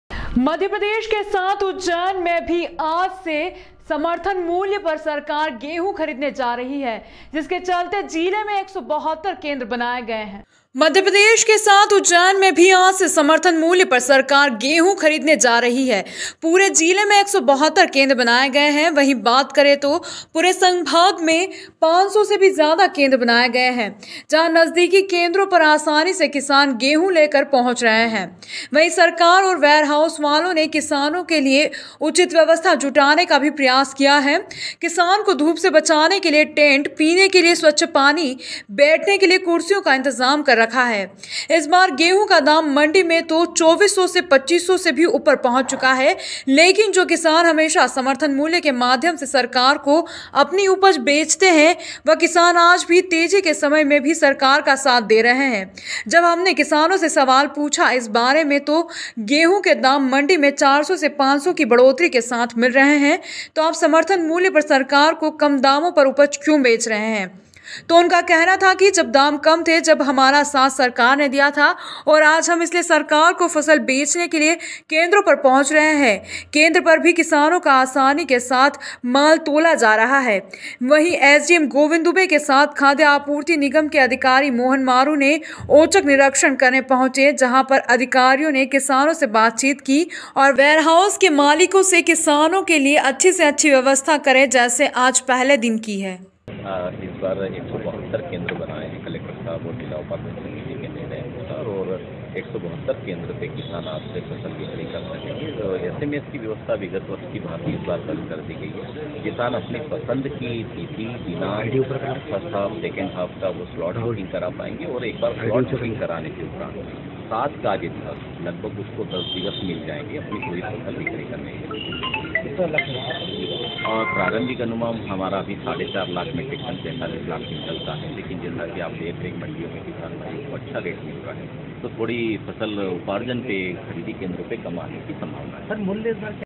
उज्जैन में भी आज से समर्थन मूल्य पर सरकार गेंहू खरीदेगी | Ujjain | Siti24ujjain | News
मध्यप्रदेश के साथ उज्जैन में भी आज से समर्थन मूल्य पर सरकार गेंहू खरीदी जा रही है पूरे जिले में 172 केंद्र बनाए गए वही बात करे तो पूरे संभाग में 500 से भी ज्यादा केंद्र बनाए गए हैं जहां नजदीकी केंद्रों आसानी से किसान पर गेहूं लेकर पहुंच रही है वहीं सरकार और वेयर हाउस वालों ने किसानों के लिए उचित व्यवस्था जुटाने का भी प्रयास किया है किसान को धूप से बचाने के लिए टेंट पीने के लिए स्वच्छ पानी बैठने के लिए कुर्सियों का इंतजाम कर रखा है इस बार गेहूं का दाम मंडी में तो  24 सो से 25 सो  से भी ऊपर पहुंच चुका है लेकिन जो किसान हमेशा समर्थन मूल्य के माध्यम से सरकार को अपनी उपज भेजते हैं वह किसान आज भी तेजी के समय में भी सरकार का साथ दे रहे हैं जब हमने किसानों से सवाल पूछा इस बार तो गेहूं के दाम मंडी में 400 से ₹500 की बढ़ोतरी के साथ मिल रहे हैं तो आप समर्थन मूल्य पर सरकार को कम दामों पर उपज क्यों बेच रहे हो तो  उनका कहना था जब दाम कम थे जब  हमारा साथ  सरकार ने  दिया था और आज हम इसलिए सरकार को   फसल  बेचने के लिए केंद्रों पर पहुंच रहे हैं केंद्र पर भी किसानों का आसानी के साथ माल तोला जा रहा है वहीं एसडीएम गोविन्द दुबे के साथ खाद्यय आपूर्ति निगम के अधिकारी मोहन मारू  ने   औचक निरीक्षण करने पहुँचे  जहा पर अधिकारियों ने किसानों से बातचीत की ओर वेयरहाउस के मालिक से किसानों के लिए अच्छी से अच्छी व्यस्था करे जैसे आप पहले दिन की है